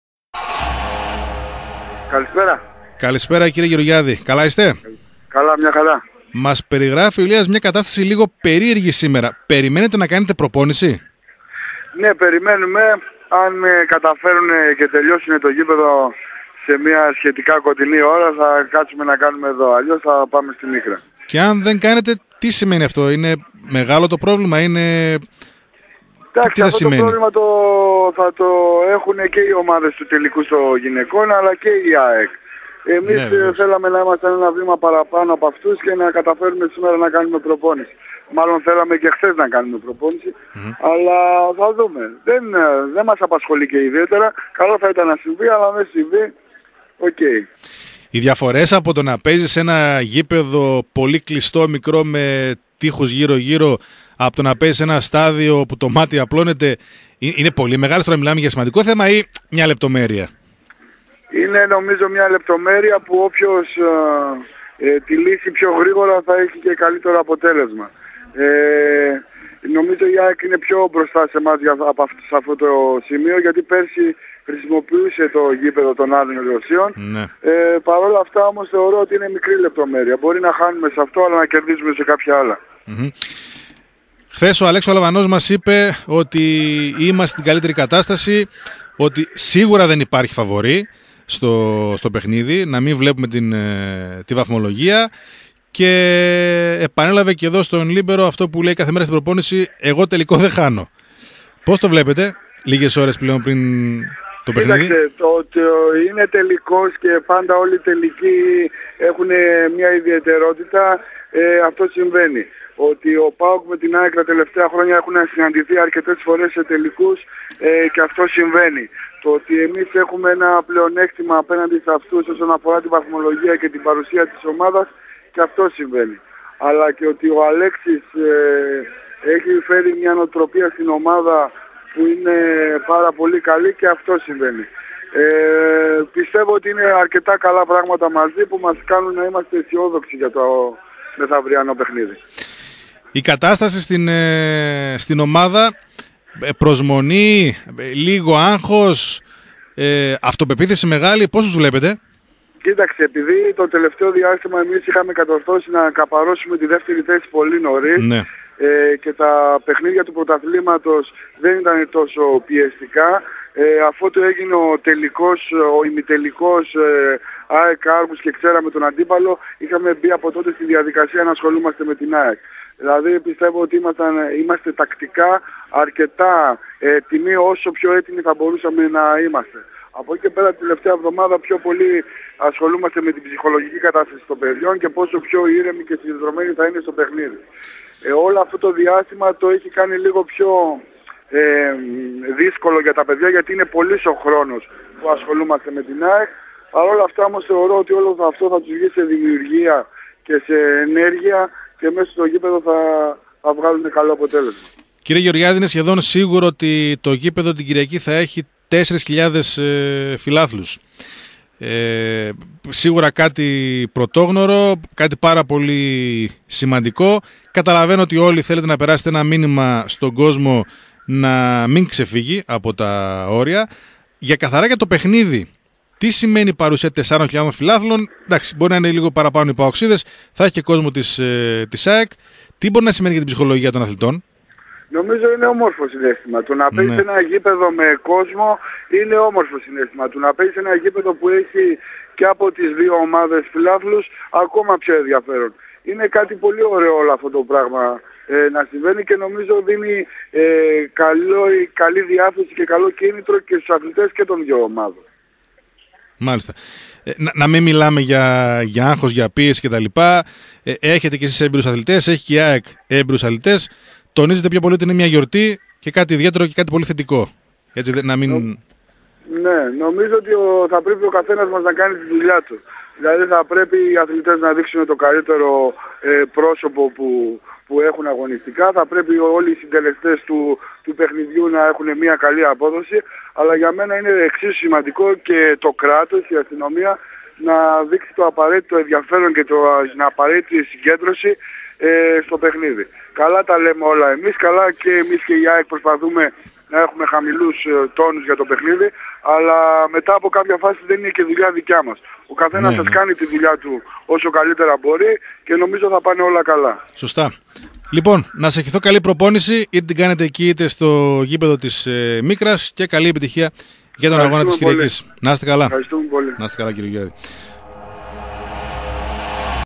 Νίκος Γεωργιάδης "Αισιόδοξοι ενόψει τελικού"
Ο προπονητής της ομάδας χάντμπολ του Stoiximan.gr ΠΑΟΚ , Νίκος Γεωργιάδης, αποκαλύπτεται στην εκπομπή του PAOK24 στον Libero 107.4 με φόντο τον τελικό Κυπέλλου.